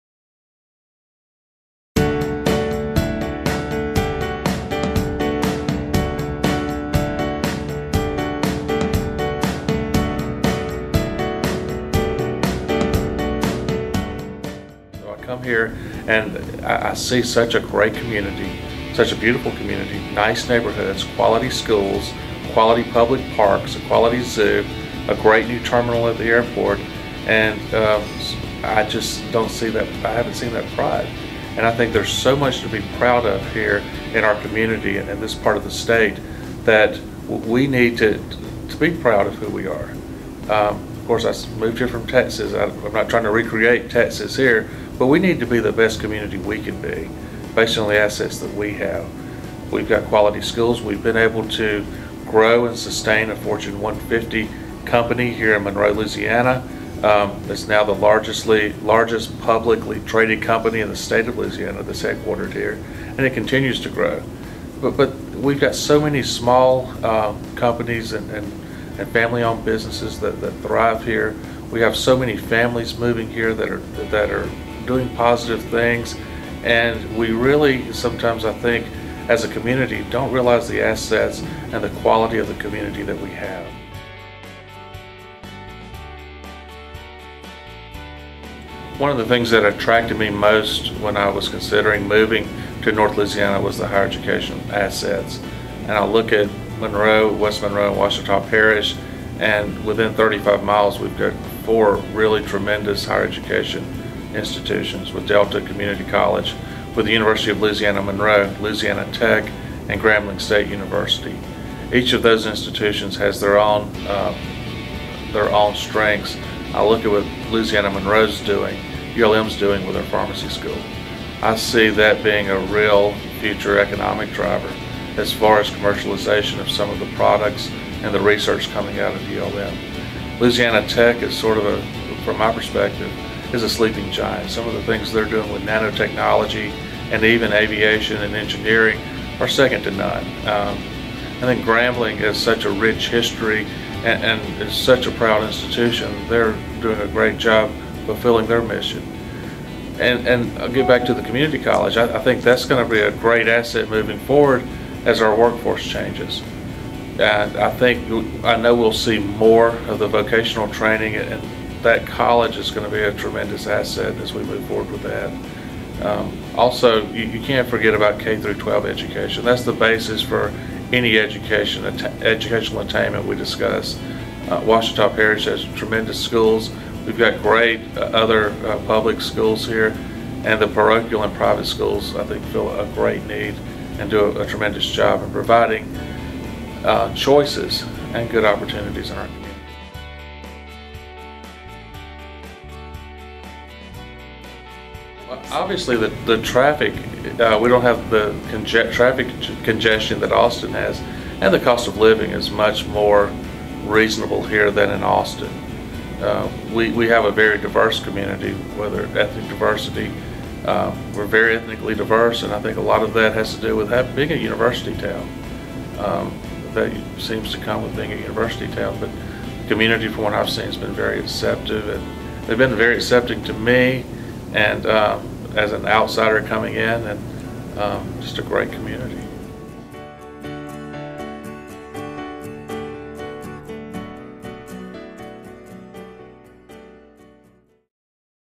Northeast Louisiana Community Pride with Scott Martinez
We sat down with Scott Martinez, President of North Louisiana Economic Partnership, and asked him 3 questions about community pride. Scott recently moved from Austin, Texas to North Louisiana after seeing all the potential the area has to offer. He encourages us to take pride in our diverse educational hub and ability to grow and sustain large corporations. Listen to why he thinks we should be the best community that we can be.

for more information about relocating, education, and housing in Monroe, West Monroe and other areas of Northeast Louisiana.